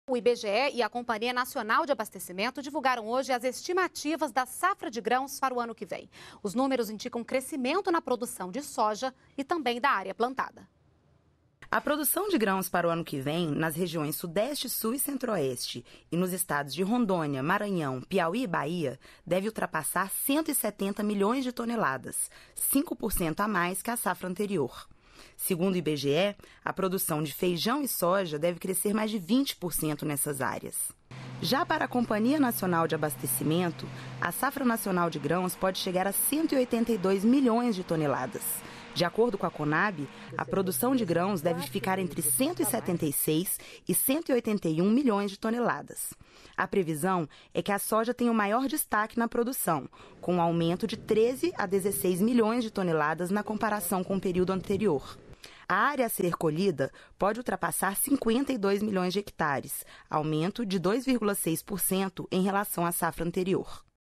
IBGE divulga estimativas da safra de grãos para 2013
NBR NOTÍCIAS - 08.11.12: O IBGE e a Companhia Nacional de Abastecimento divulgaram nesta quinta-feira (09) as estimativas da safra de grãos para 2013. Os números indicam crescimento na produção de soja  e também da área plantada.